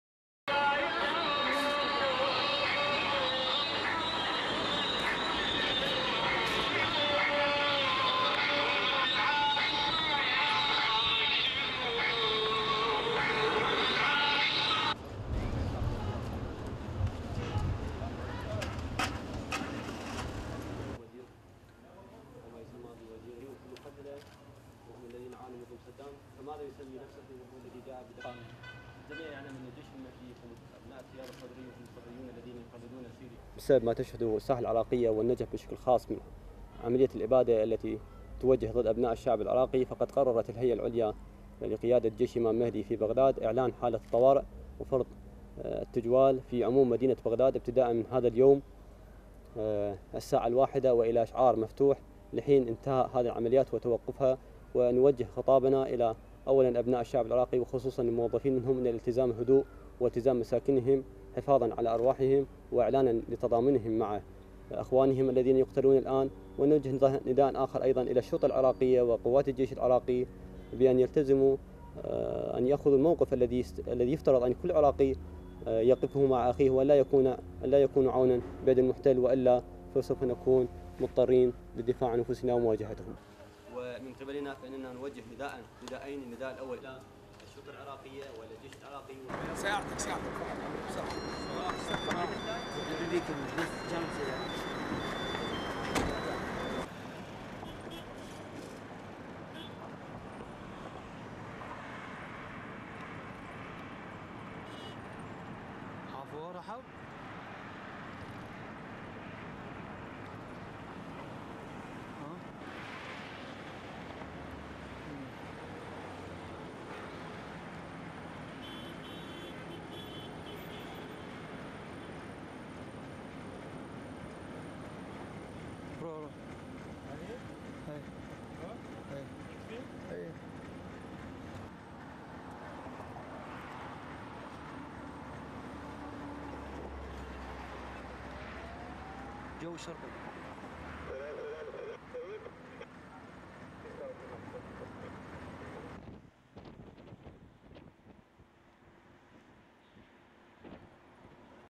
Burning Humvee, al-Sadr spokesman comment
1. Wide of streets in Sadr City
2. Exterior of Sadr office
3. Exterior of Imam al-Sadr mosque (named after father of Muqtada al-Sadr)
4. Journalists at press conference
5. Armed bodyguard
6. Photographers
7. SOUNDBITE (Arabic) Qais al-Ghazali, Al-Sadr spokesman:
"Because of what is happening in Iraq and Najaf, this genocidal war against the people of Iraq, the leadership of the al-Mahdi Army decided to declare a state of emergency and a curfew all over the city of Baghdad, starting at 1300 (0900 GMT). It will continue until this aggression stops. We directed these comments to the people of Iraq and the civilian workers to stay at home to save their lives and to give the solidarity to those who have died and for their brothers who have been fighting in recent days. We call upon the Iraqi Army and Iraqi Police to take an honourable stance that each and every Iraqi should take. They should not be tools in the hands of the invaders. If they do, we will be forced to defend ourselves and Islam by challenging them."
8. Wide of press conference
9. Wide of smoke from road, from a humvee burning in east of Baghdad
10. Various of humvee burning
STORYLINE:
A spokesman for militant cleric Muqtada al-Sadr called on workers in Baghdad to observe a curfew from 1300 local time (0900 GMT).
The call comes after several days of fighting in the city.
Al-Sadr spokesman, Qais al-Ghazali, told reporters Tuesday that Iraqis and civilian workers should stay at home and support the militants who have been fighting in heavy battles against coalition forces in the last few days.
Al-Ghazali also warned the Iraqi Army and Iraqi Police to "not be tools in the hands of the invaders."
The al Mahdi Army militiamen in Baghdad kidnapped a senior Iraqi policeman, Brigadier Raed Mohammed Khudair, who is responsible for all police patrols in eastern Baghdad, said Colonel Adnan Abdel Rahman, an Interior Ministry spokesman.
In a video broadcast on the pan-Arab network Al-Jazeera, militants said the government should release all al Mahdi Army prisoners in exchange for Khudair.
The Iraqi government on Monday imposed a night time curfew in Baghdad's mainly Shi'ite Muslim neighbourhood of Sadr City.
Also in Baghdad, a US  humvee was seen burning by the roadside close to the former UN headquarters.
It was not known what caused the fire or if there were any injuries, but humvees and other military vehicles regularly come under attack from militants around the Iraqi capital.